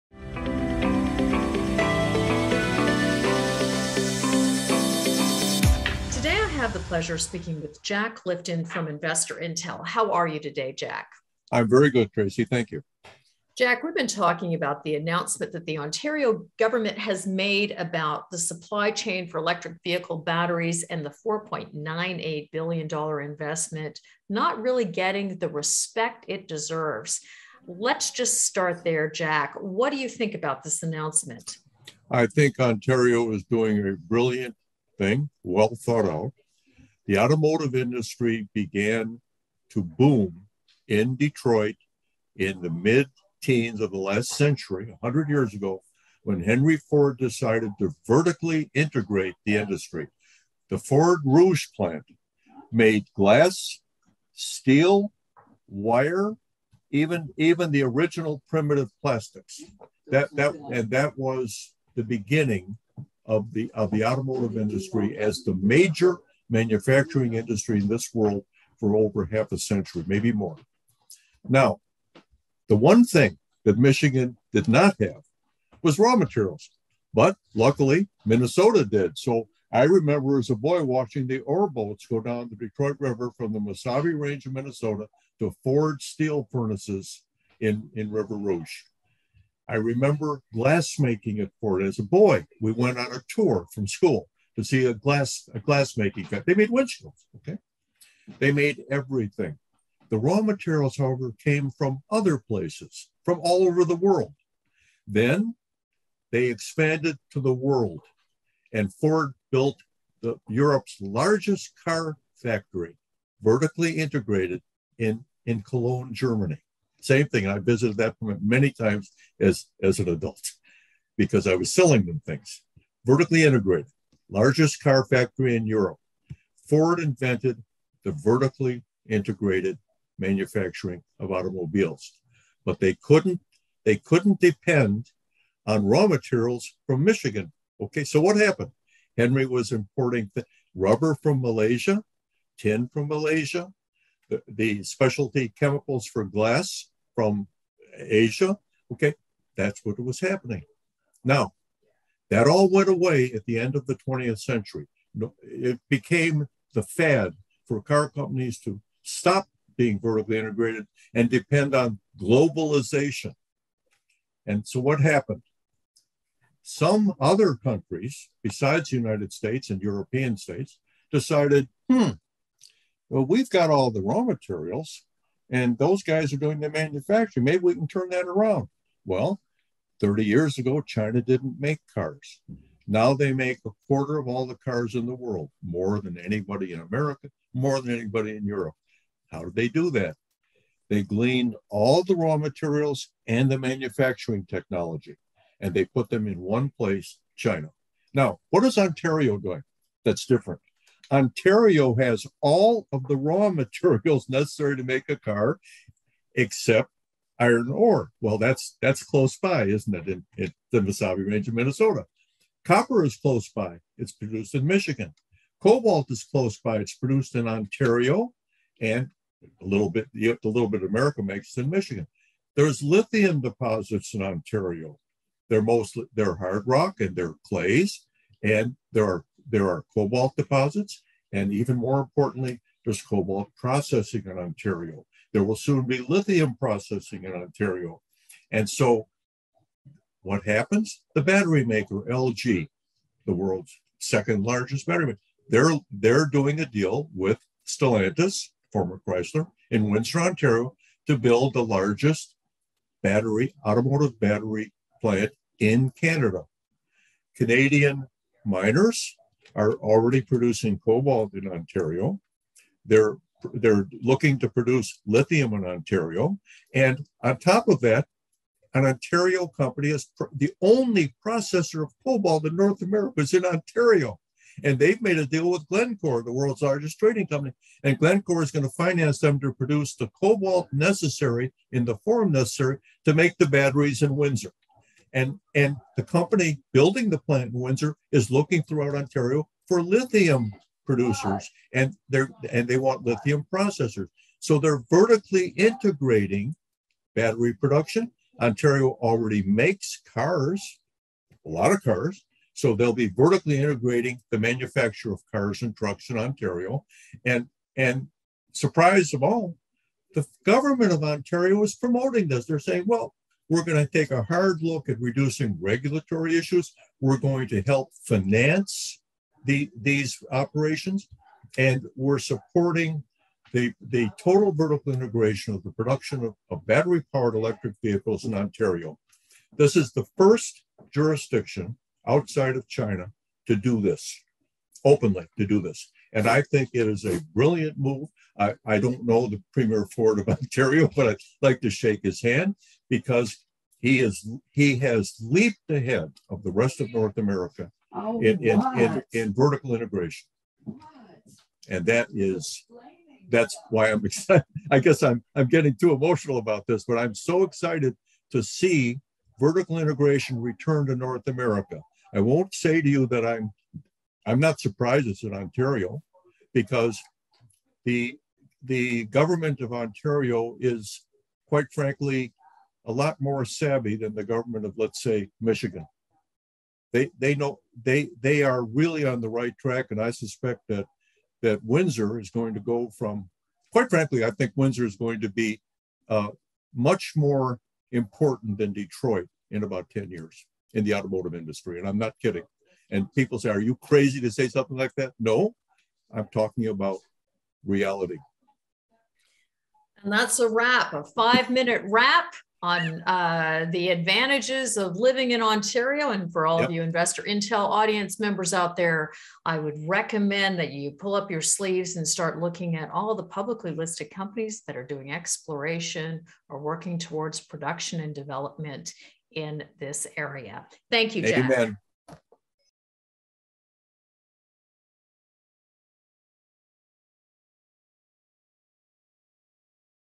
Jack Lifton on how the Windsor battery plant marks “the return of total vertical integration to NA”
In this InvestorIntel interview, Tracy Weslosky is joined by Critical Minerals’ industry expert and InvestorIntel Editor-in-Chief Jack Lifton to discuss the Ontario government’s recent announcement to make the largest private sector investment in Ontario history in a $5B Windsor battery plant.

Jack discusses the cyclic history of vertically integrated OEM automobile manufacturing in the US and Canada and its decline, due to globalization by the domestically owned US automotive manufacturing industry. Jack sees an imminent return to the industry of vertical integration, first in Canada’s automotive manufacturing center, Ontario. Jack explains how the Windsor battery plant in fact marks “the return of total vertical integration to North America” making Windsor, perhaps, as a symbol of Ontario’s natural critical resources and manufacturing capacities possibly “more important than Detroit in about 10 years in the North American OEM automotive industry.”